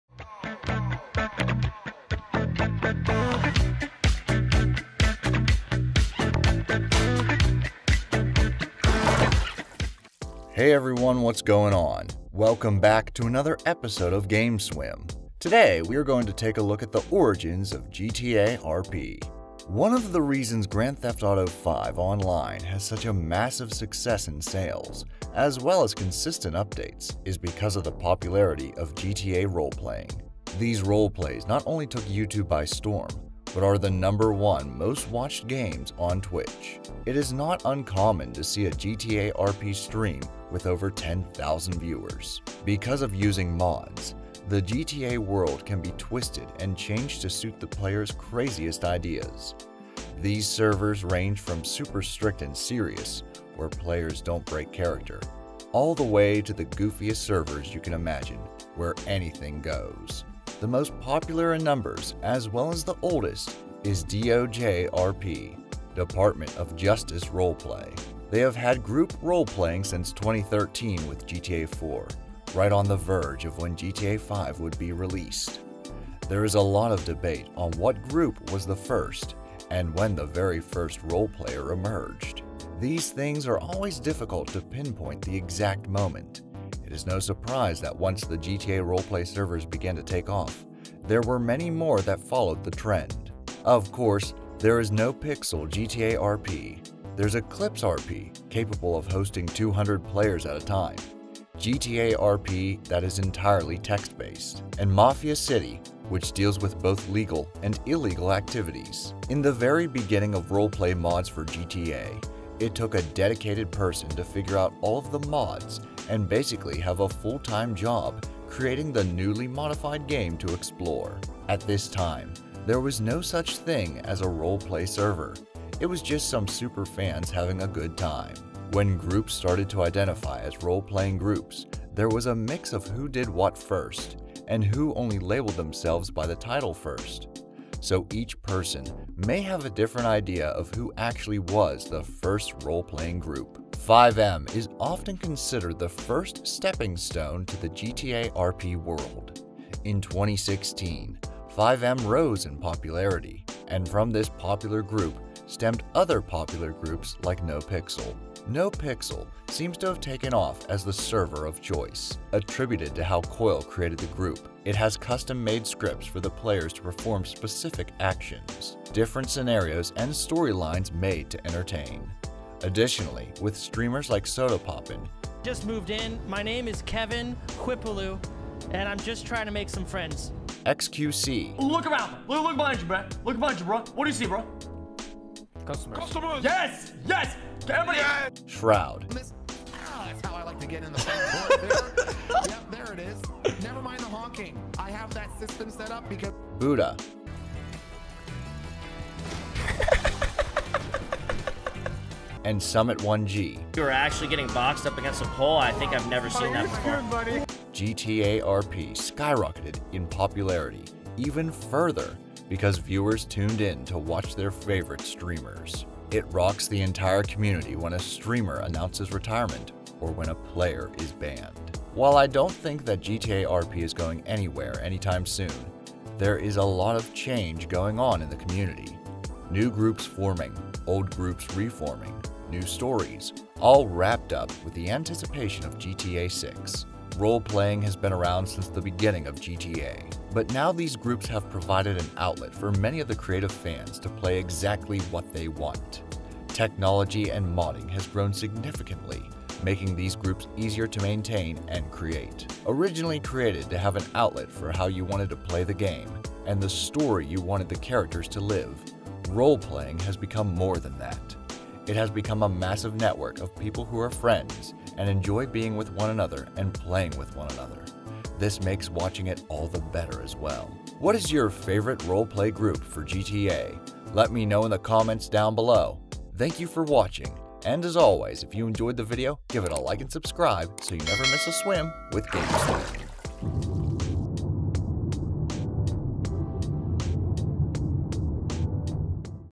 Origins Of GTA RP
Hey everyone, what’s goin’ on? Welcome back to another episode of Game Swim. Today we are going to take a look at the origins of GTA RP and how it all started.

Thank you all so much for your continued support and belief in the channel!